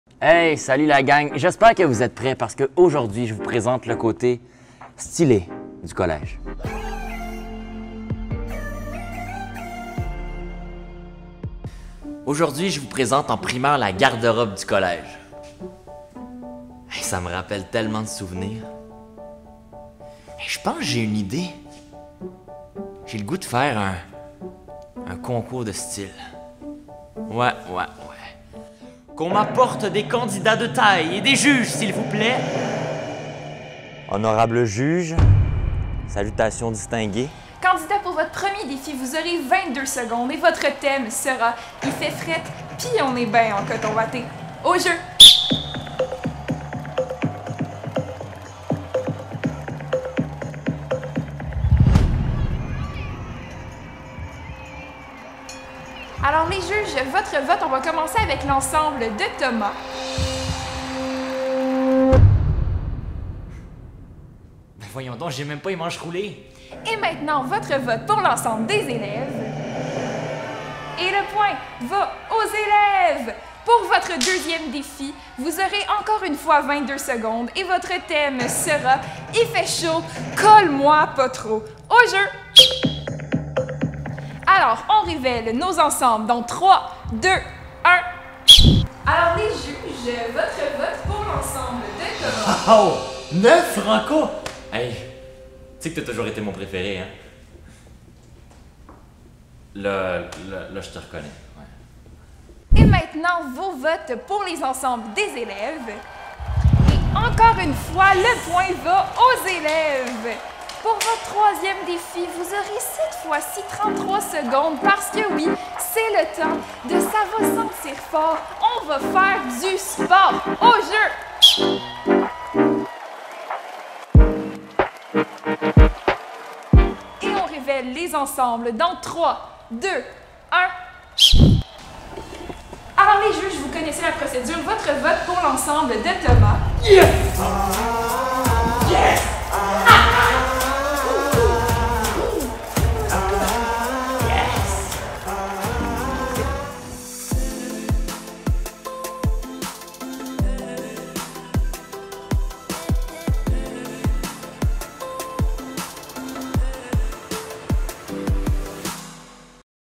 Épisode 2 | La tenue vestimentaire
La tenue vestimentaire est un élément distinctif et rassembleur au Collège. Pour la découvrir, nous vous proposons d'être les témoins d'un défilé de mode hors du commun.